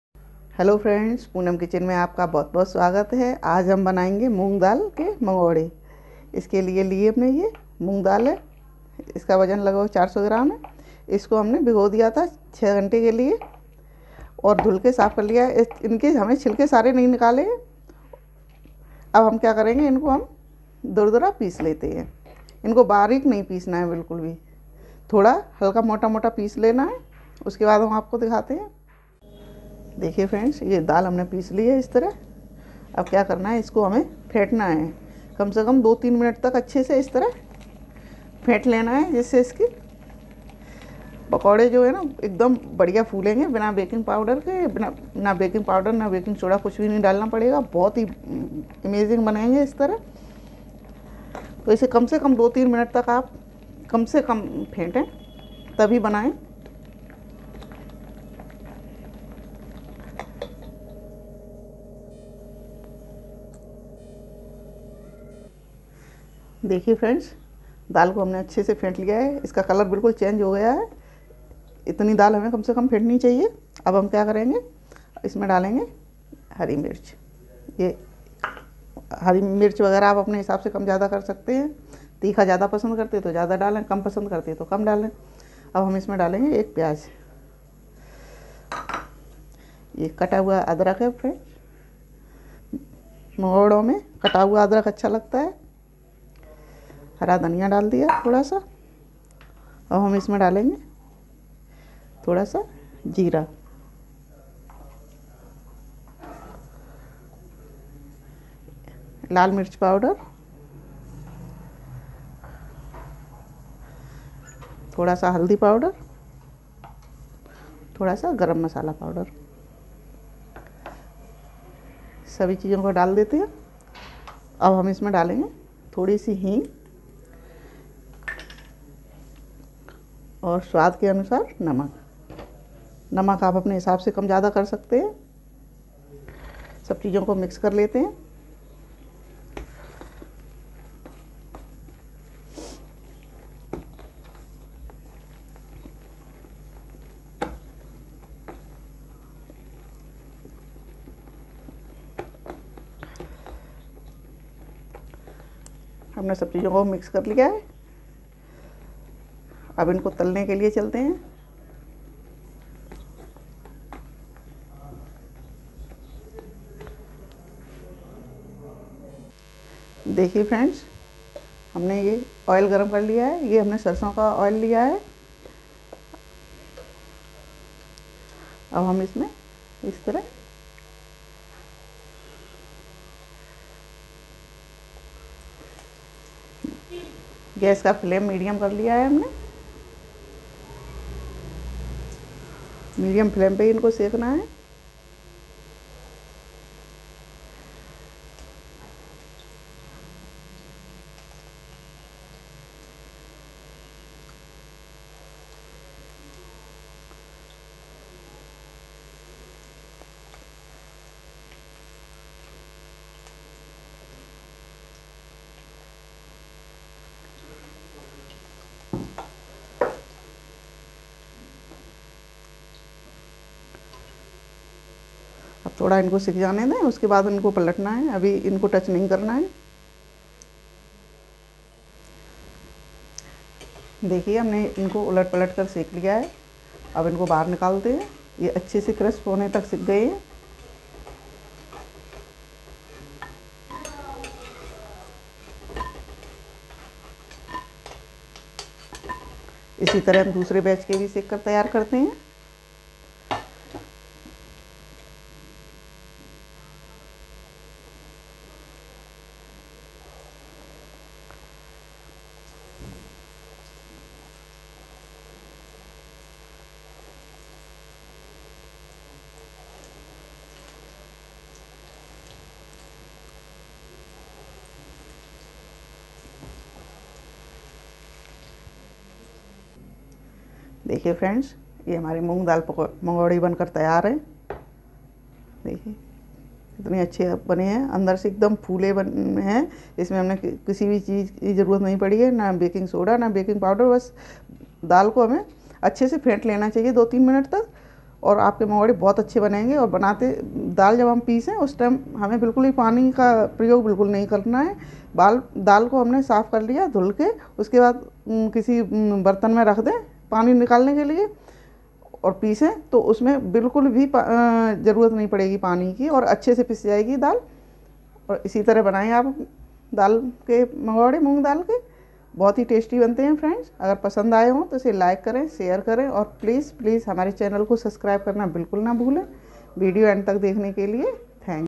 ना समोसा ना कचोरी मूंग दाल के मंगोड़े मेहमान आ जाए घर पर तो बताना मत रेसिपी ll New Recipe
This is the recipe of how to make tasty and delicious evening snacks.